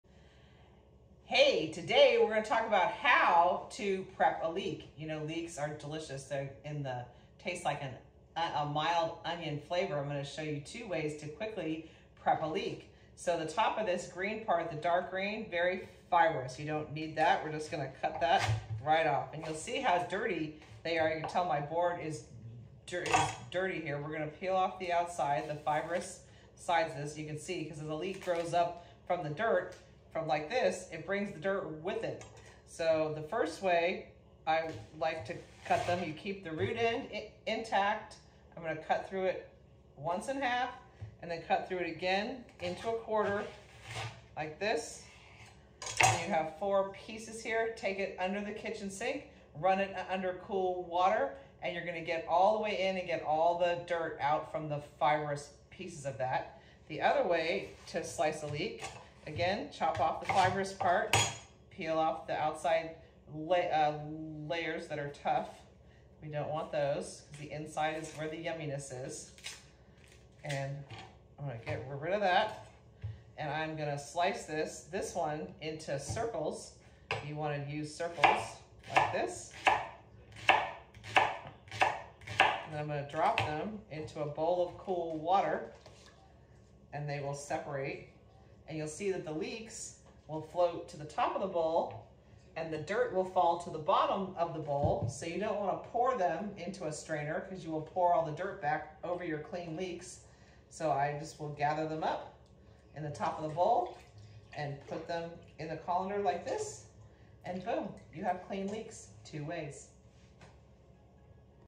How to Clean a Leek 2 ways Step by Step
Leeks can be very sandy and dirty so it's important you get them clean.  Here's two easy way to clean them!

Get our FREE resources!

FREE Summer Dinner Party Challenge with How To Videos, Menu and Timeline (Elote Chicken Tacos and more!)